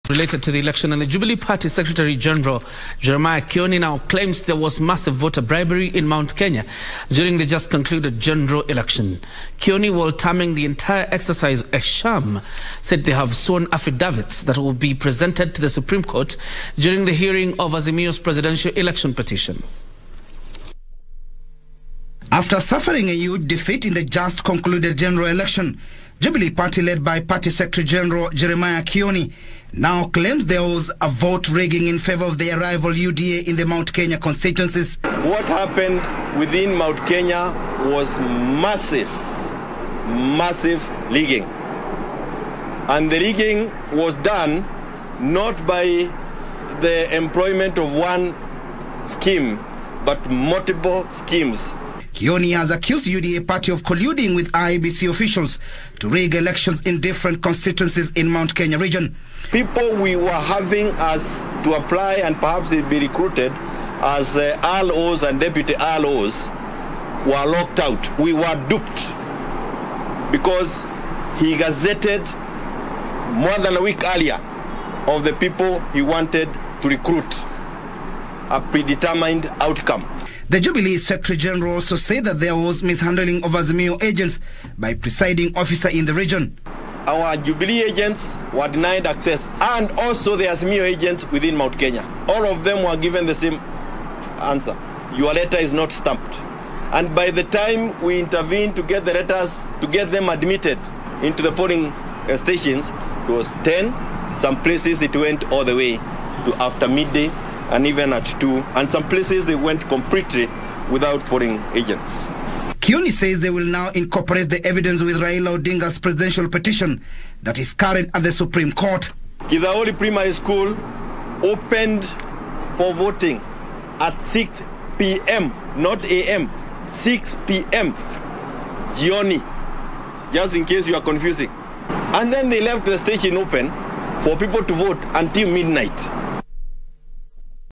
Jubilee claims massive rigging in the central region
Jubilee Party Secretary General Jeremiah Kioni now claims there was massive voter bribery in Mount Kenya during the just concluded general election.  Kioni, while terming the entire exercise a sham  said they have sworn affidavits that will be presented to the Supreme Court during the hearing of Azimio’s presidential election petition.